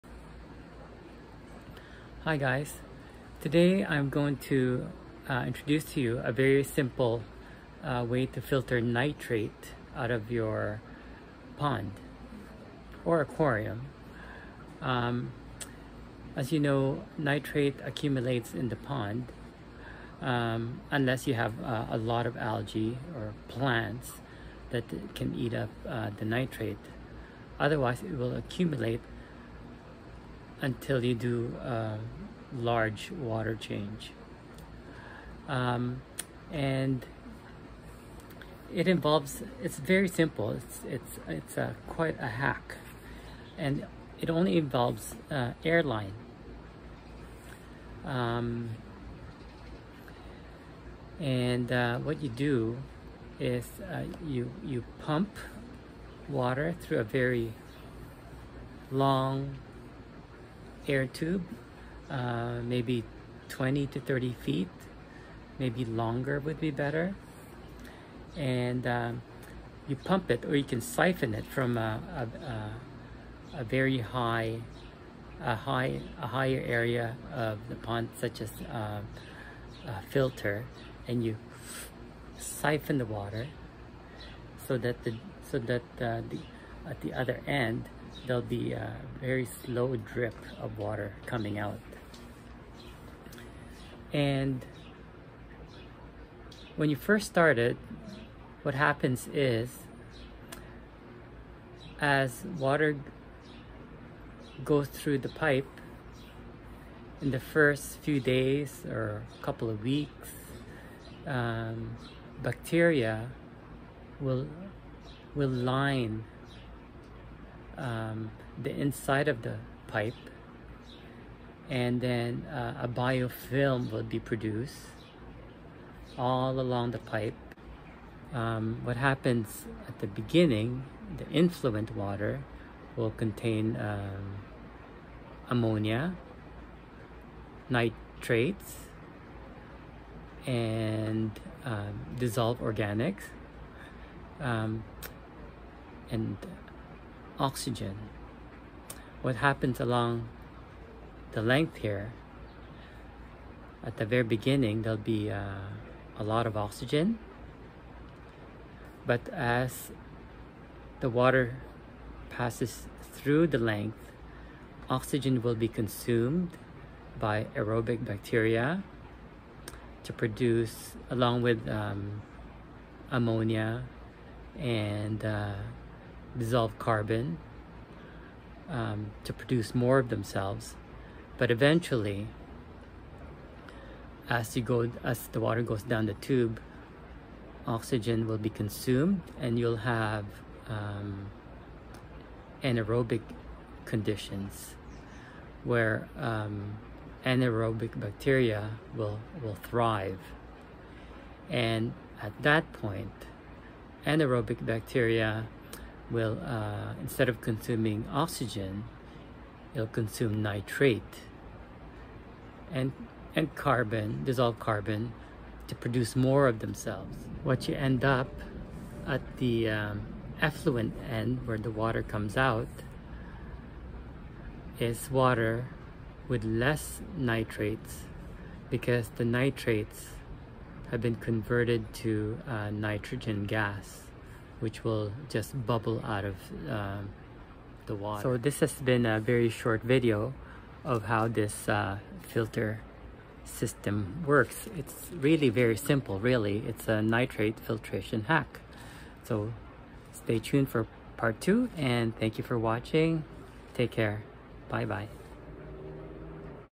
Nitrate filter hack, introduction, part 1, koi pond
Please join me on my journey on my quest to reduce nitrates in my pond using this nitrate filter hack, it’s application and ways to improve it.